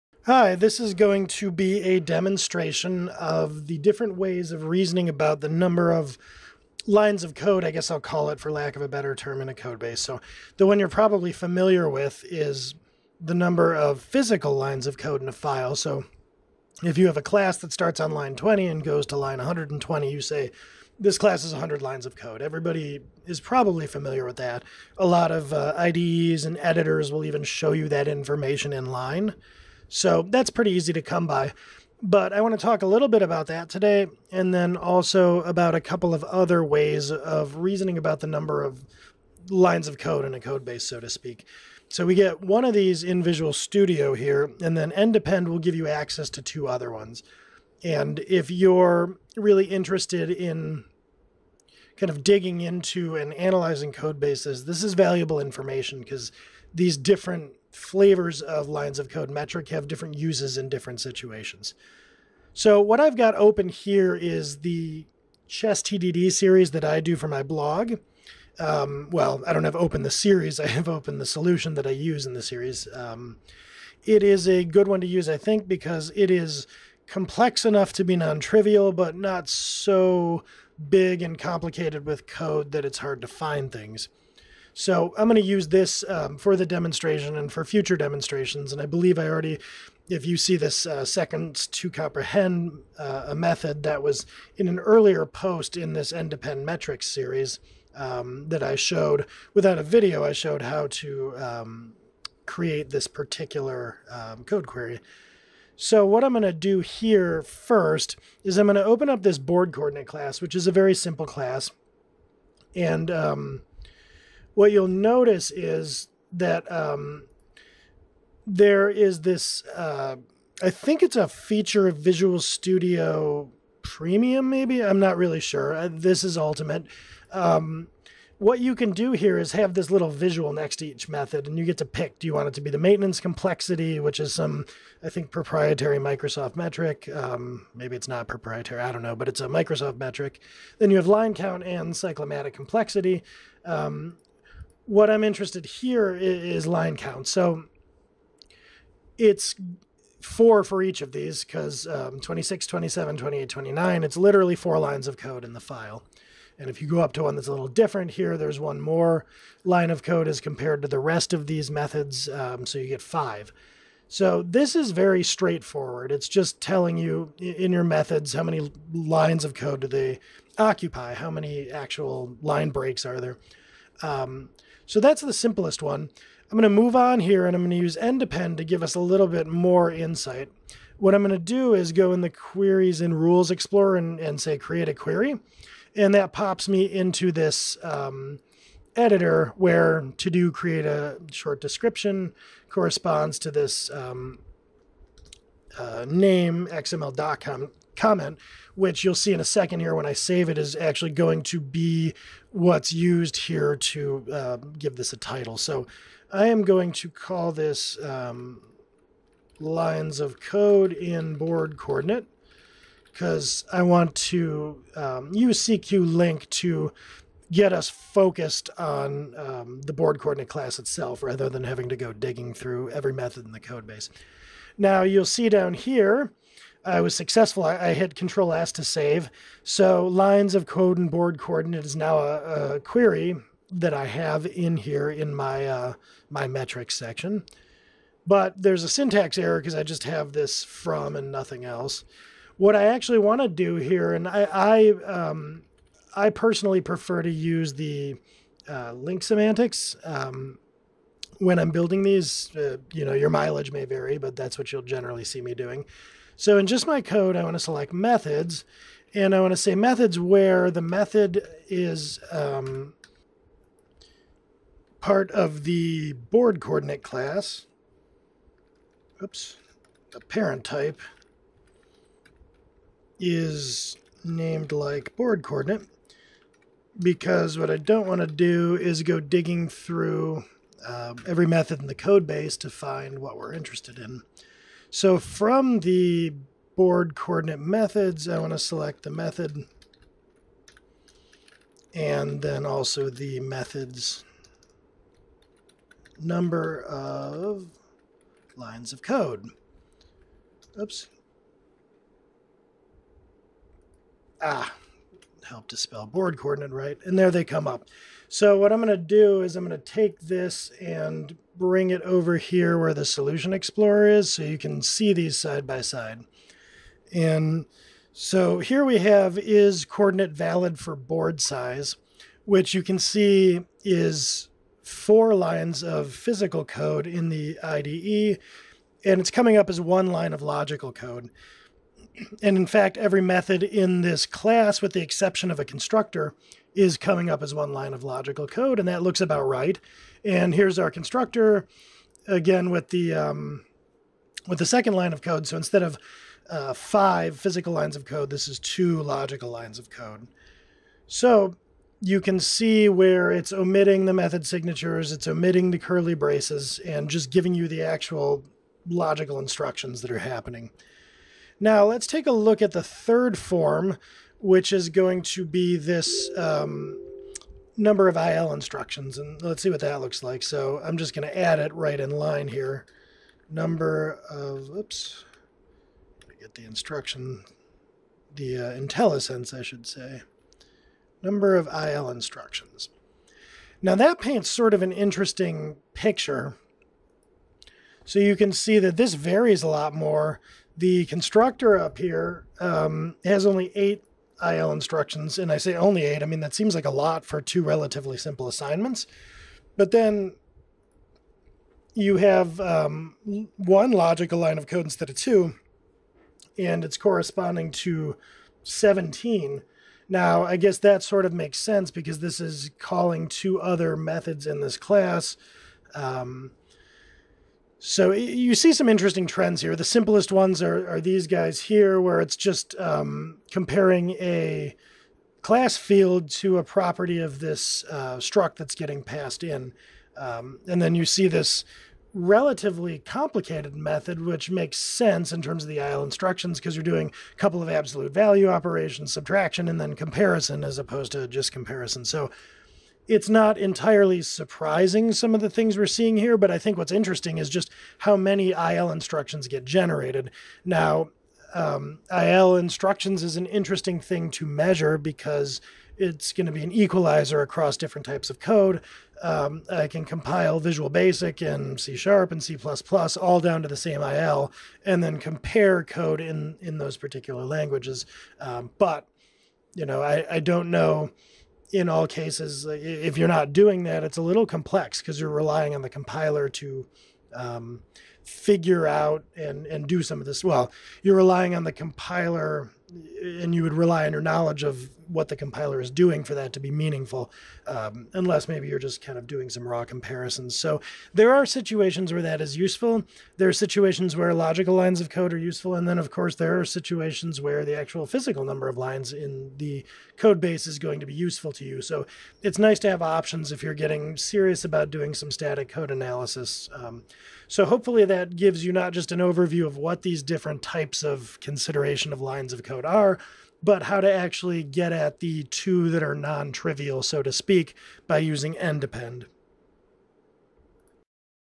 Different Lines of Code Metrics with NDepend
You know about physical lines of code, obviously.  Tune in to see what logical lines of code and number of IL instructions look like and how to calculate them.